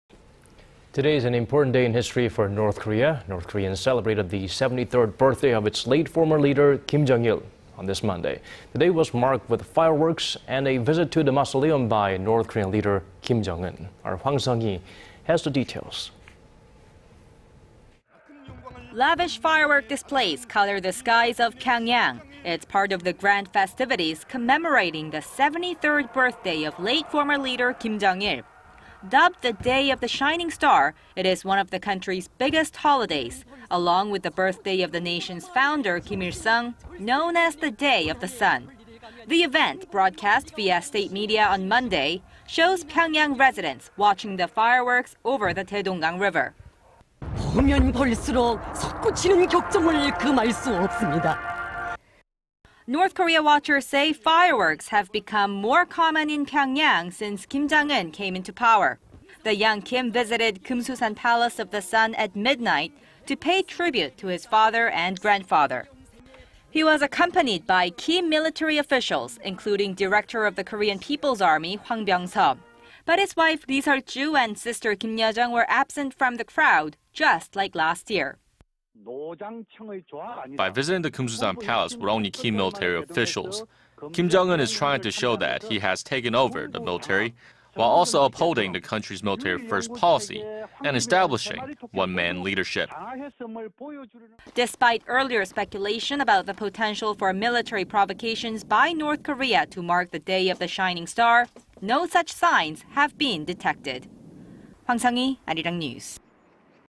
North Korea celebrates 73rd birthday of late former leader with lavish festiviti
North Korea celebrated the 73rd birthday of its late former leader Kim Jong-il on this Monday.
The day was marked with fireworks and a visit to the mausoleum by North Korean leader Kim Jong-un.
Hwang Sung-hee reports.
Lavish firework displays color the skies of Pyongyang.
It′s part of the grand festivities commemorating the 73rd birthday of late former leader Kim Jong-il.
Dubbed the "Day of the Shining Star," it is one of the country′s biggest holidays, along with the birthday of the nation′s founder Kim Il-sung, known as the "Day of the Sun."
The event, broadcast via state media on Monday, shows Pyongyang residents watching the fireworks over the Taedong-gang river.

"We cannot hold back our growing passion."
North Korea watchers say fireworks have become more common in Pyongyang since Kim Jong-un came into power.
The young Kim visited Kumsusan Palace of the Sun at midnight... to pay tribute to his father and grandfather.
He was accompanied by key military officials, including Director of the Korean People′s Army Hwang Pyong-so.
But his wife Ri Sol-ju and sister Kim Yo-jong were absent from the crowd, just like last year.

"By visiting the Kumsusan Palace with only key military officials, Kim Jong-un is trying to show that he has taken over the military while also upholding the country′s military-first policy and establishing one-man leadership."
Despite earlier speculation about the potential for military provocations by North Korea to mark the "Day of the Shining Star," no such signs have been detected.